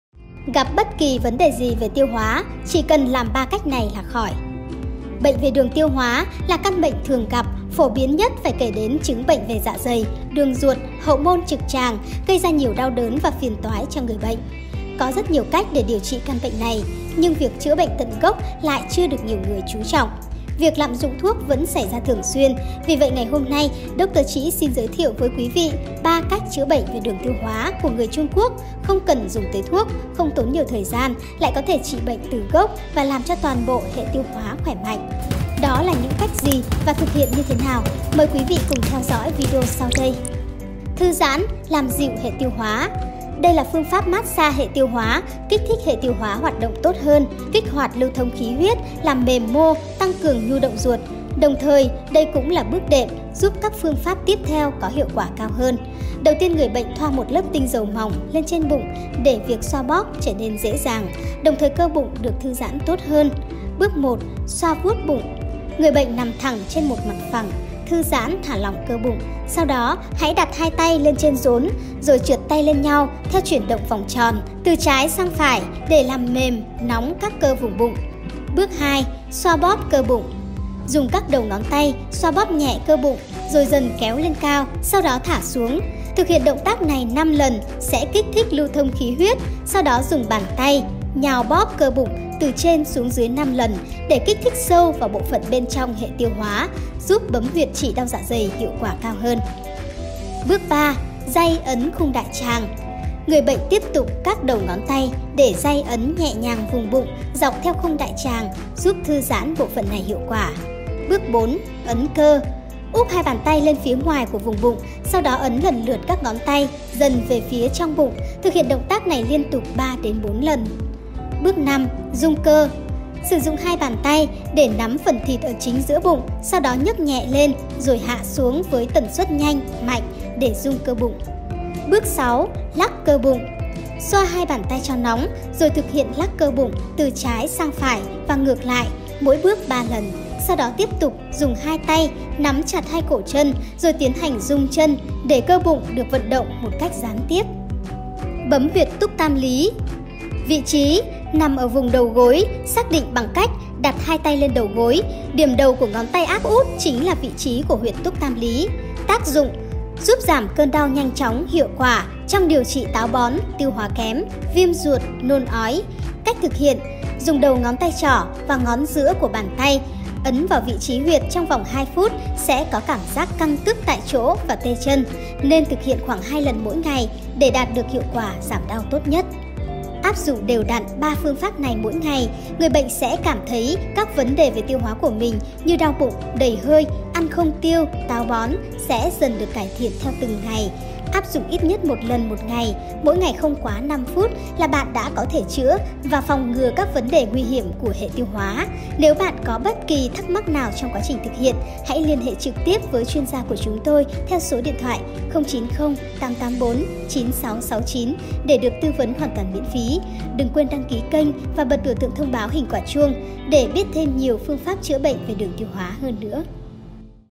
ĐỐC TƠ TRĨ: MẸO CHỮA BỆNH TRĨ BẰNG RAU SAM MỌC DẠI NGAY TRONG VƯỜN
Rau sam là một loài cỏ dại mọc ở khắp nước ta, chúng có tính hàn, vị chua mặn, thường được dùng để ăn sống, nấu canh hoặc luộc. Đây cũng là loại rau dại xuất hiện thường xuyên tại các bữa ăn của các gia đình Việt tại vùng nông thôn với giá thành rẻ và vô cùng dễ kiếm.